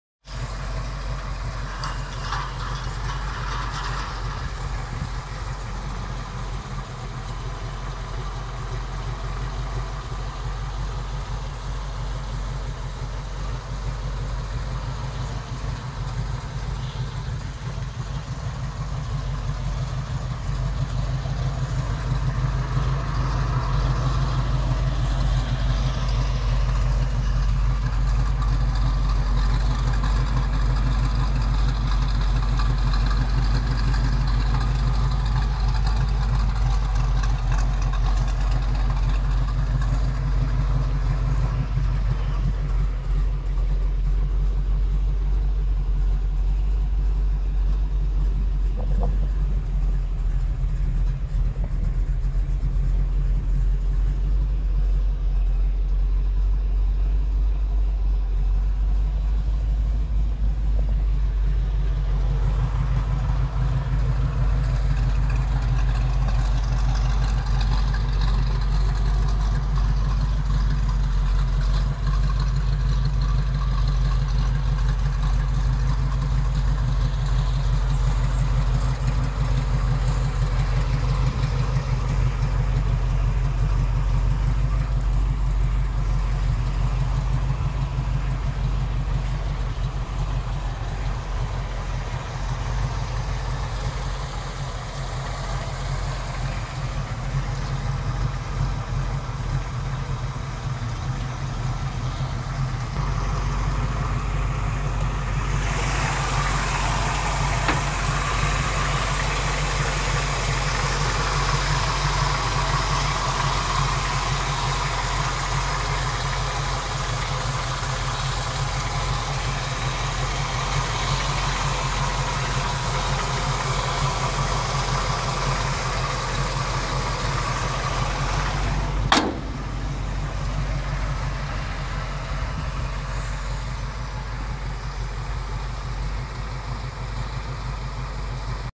68 RS Camaro walk around @
Very nice Camaro and awesome driver.
RS package with lots of cool options